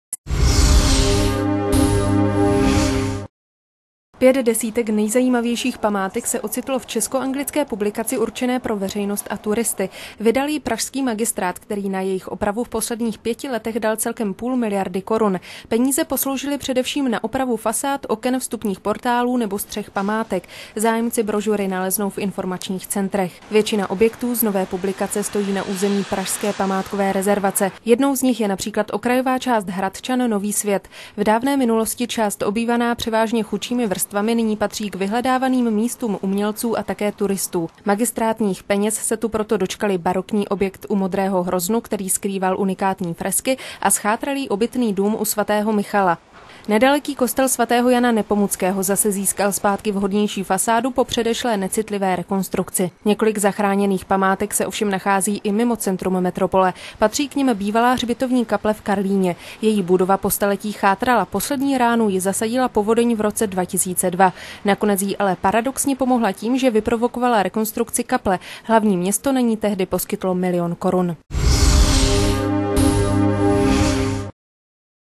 Padesát nejzajímavějších památek do kapsy
Praha - Pět desítek nejzajímavějších památek se ocitlo v česko-anglické publikaci určené pro veřejnost a turisty.Vydal ji pražský magistrát, který na jejich opravu v posledních pěti letech dal celkem půl miliardy korun. Peníze posloužily především na opravu fasád, oken, vstupních portálů nebo střech památek. Zájemci brožury naleznou v informačních centrech.